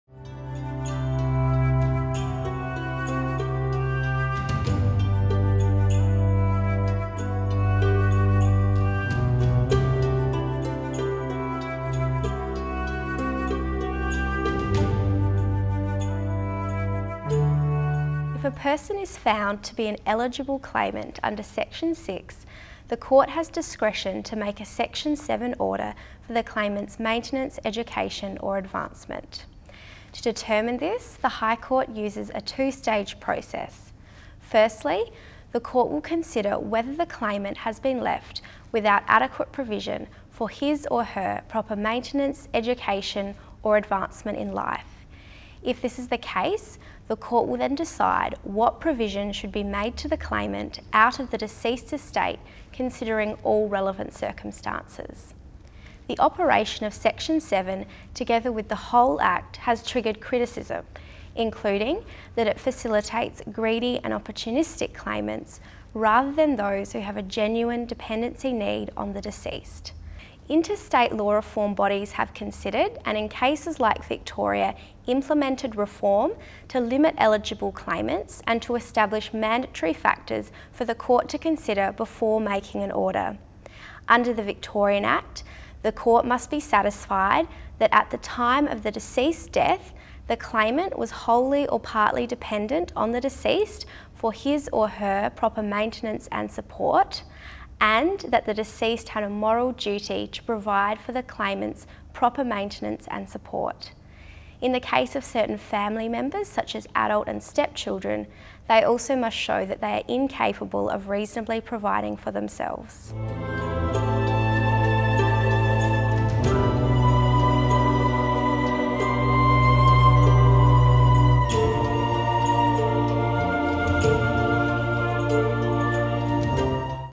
SALRI Video Fact Sheet 5: What other criteria should apply?
The South Australian Law Reform Institute (SALRI) wants your views on whether the current laws that govern who can contest a will when a family member dies are working fairly or should be changed.

Should court be able to change who gets property after a person dies, even when they leave a clear will?  Should adult children be able to make a claim that they didn’t receive a fair share of their parent’s estate?  These are some of the questions we want to hear your thoughts on. to be involved.